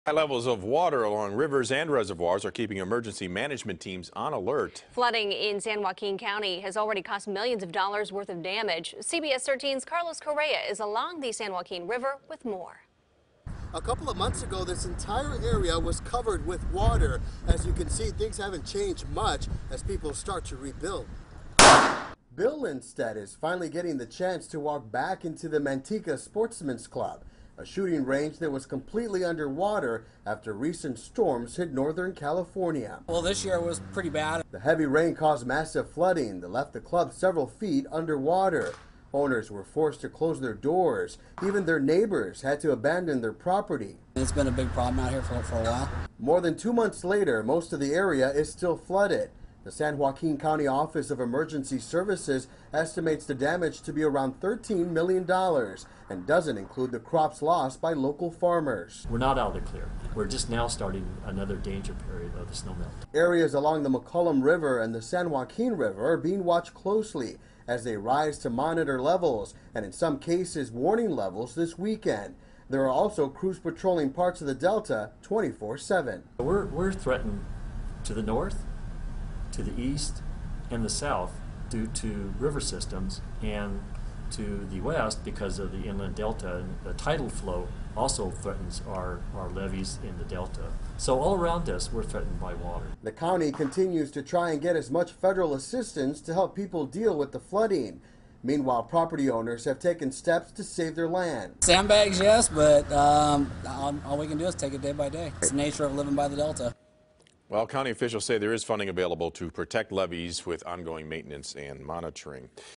San Joaquin County River Levels Expected To Remain High
High water levels along rivers and in reservoirs are keeping emergency management teams on alert after winter storms already left damage.